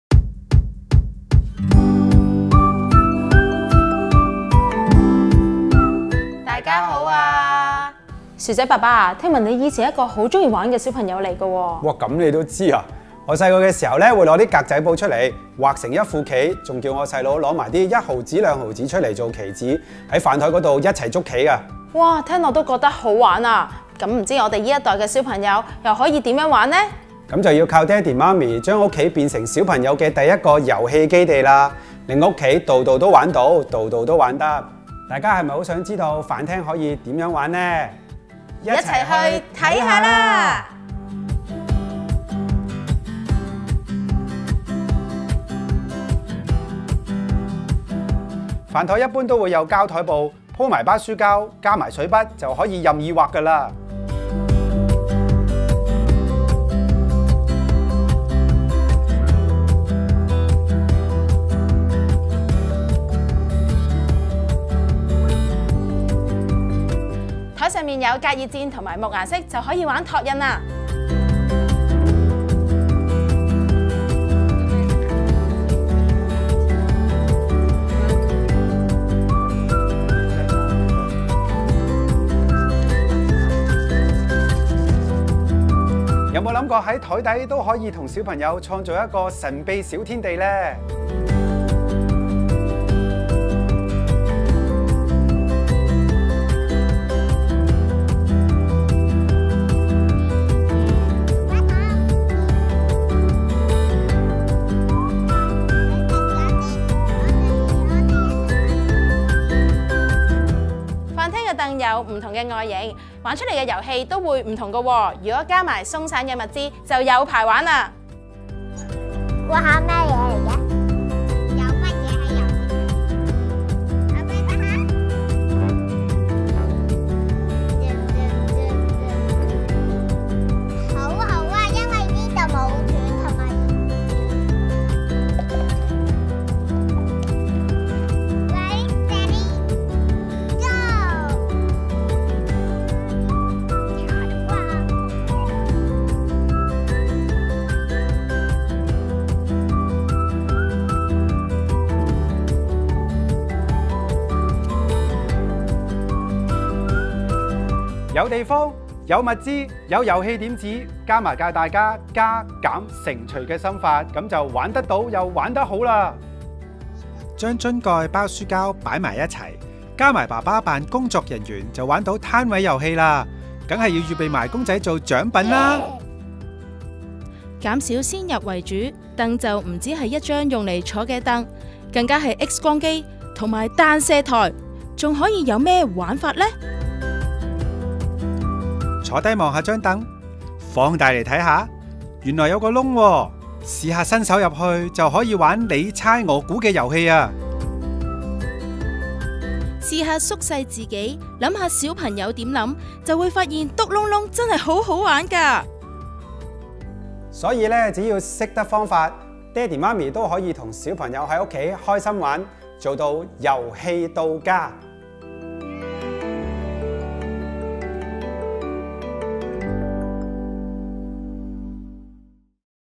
抗疫在家——智樂施展心法，飯廳也可變好玩！
面對第五波的嚴峻疫情，家長和小朋友都要避免外出，留家抗疫；那麼，如何在家中創造遊戲？在智樂好玩爸媽的眼裏，其實，家中處處都可見遊戲，處處也可玩！就來看看飯廳如何變出不同遊戲，兼學懂何謂「加減乘除」的心法，就一定玩得到，又玩得好！